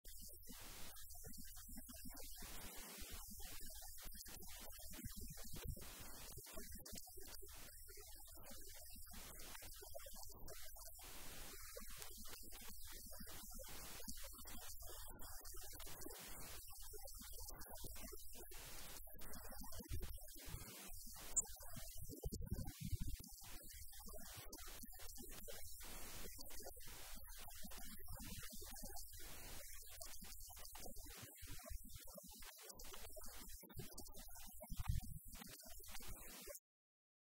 Aaska Wariye Maxamed Ibraahin Gabow oo shalay Muqdisho Lagu Dilay
Waa aaskii wariyihii TVga Kalsan oo shalay lagu dilay xaafadda wadajir. Allaha u naxariisto - aamiin